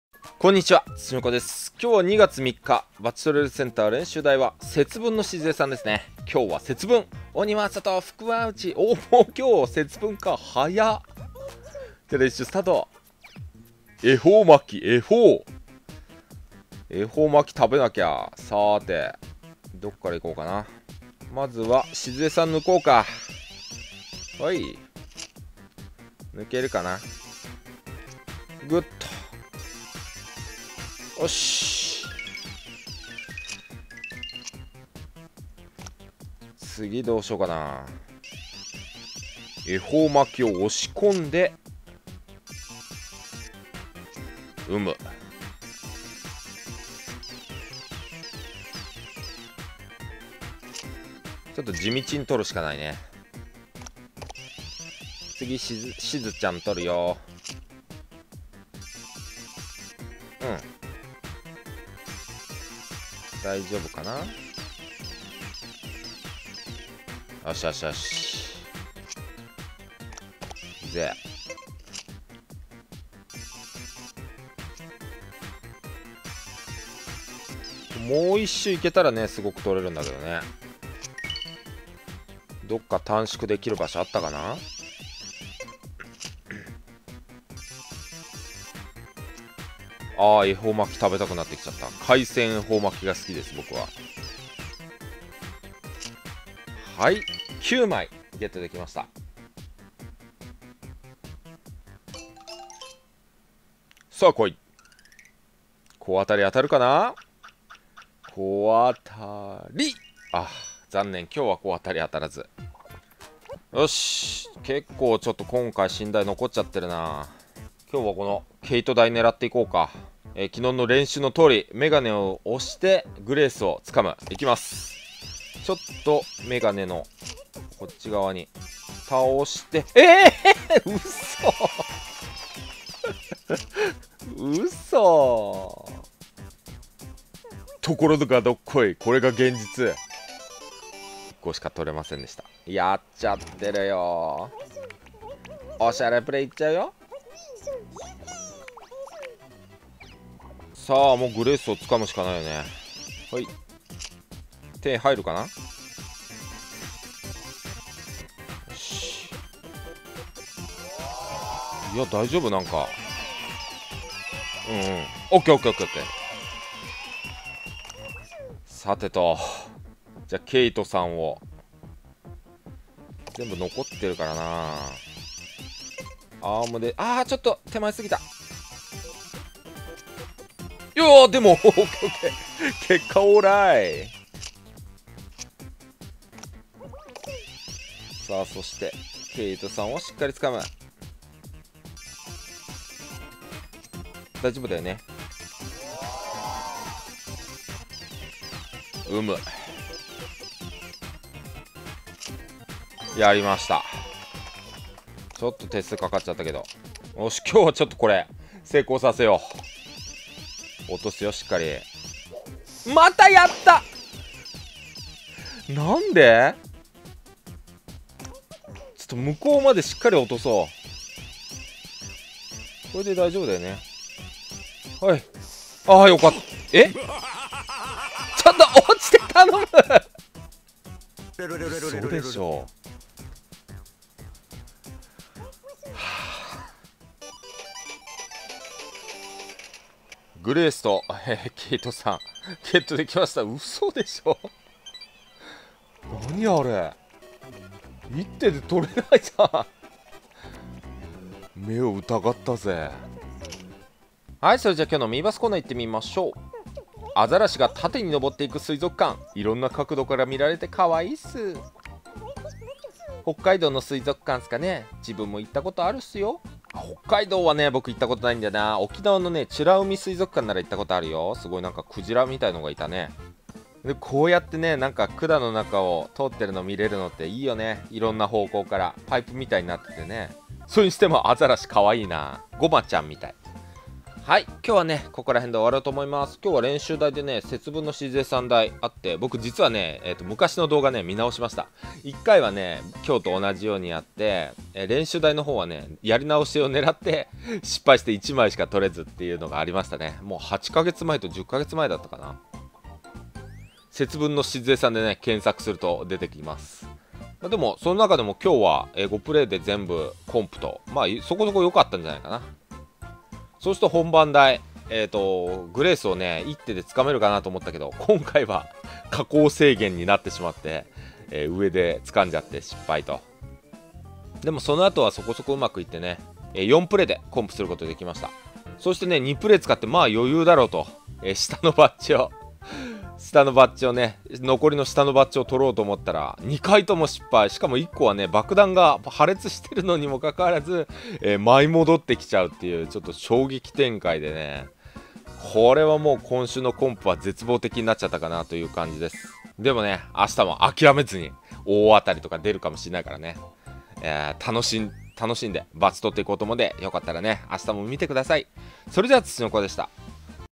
【2/3】うそだろ…爆弾が効かない… バッジとれーるセンター実況 節分のしずえさん練習台・ケイト台・ハムスタージミー台
今日のバッジとれ～るセンター練習台は節分のしずえさん台、5プレイで全部取れる感じだったよ♪本番では昨日練習したグレース掴みをやったりのこり一つのジミーを狙ったりしたけど、衝撃展開だったぜ・・・